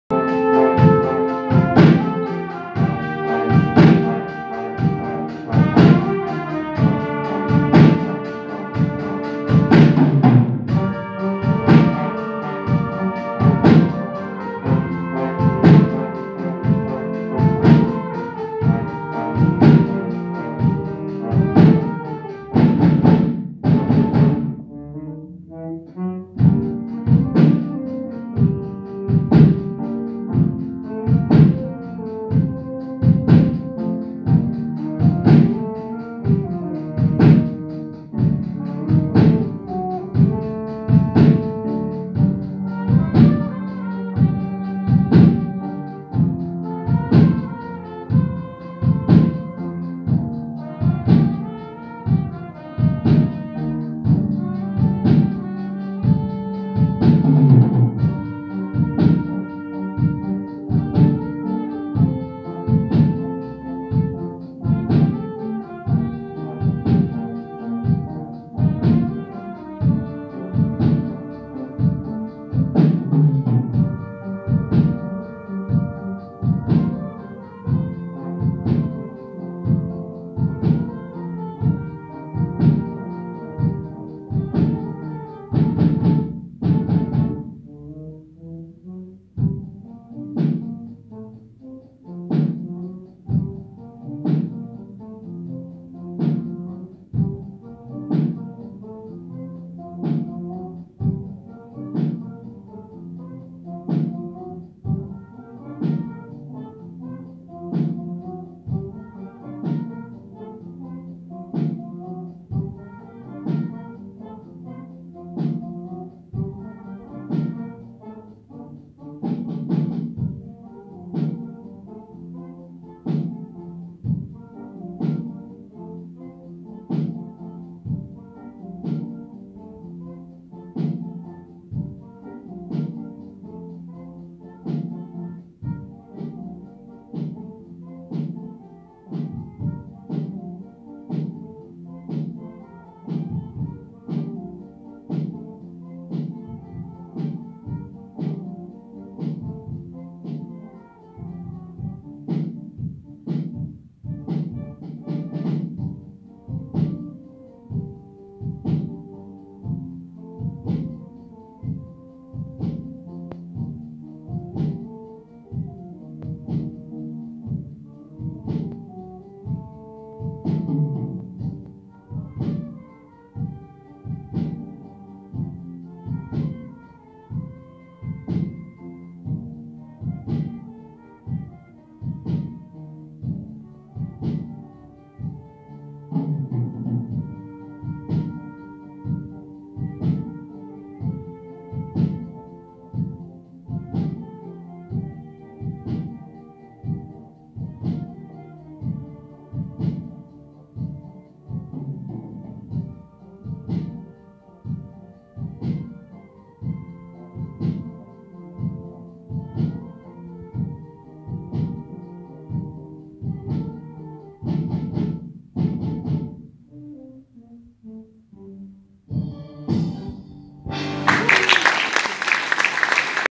Loudoun Academy Jazz Band - Young Musician of the Year 2018
My son playing with his friends in the school jazz band, and winning :)